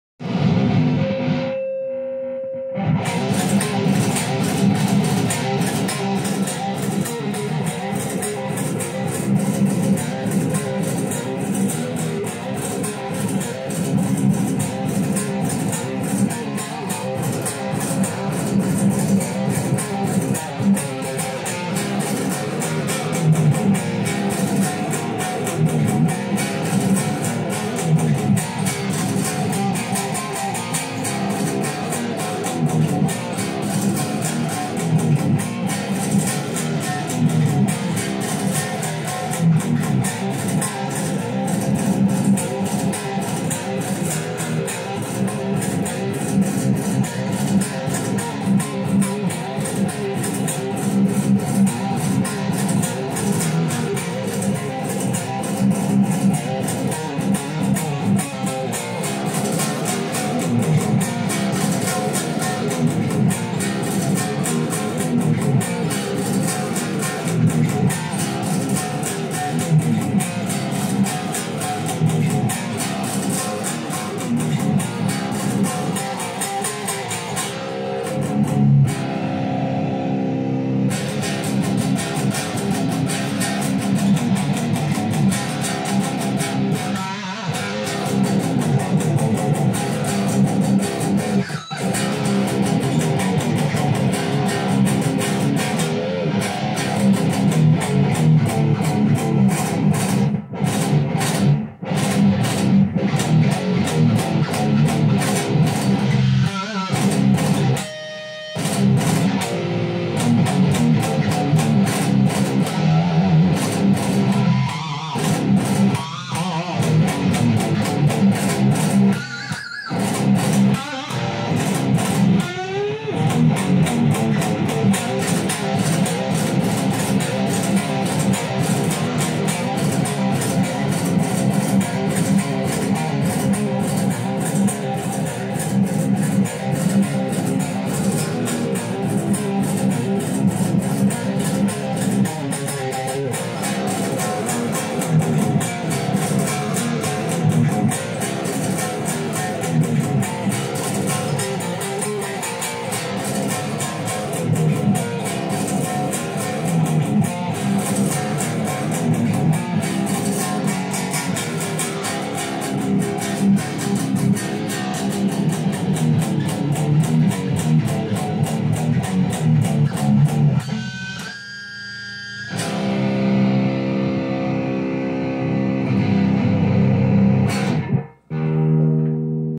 Tribute to Hatebreed.
Short few original metal riffs in tribute to the great Hatebreed.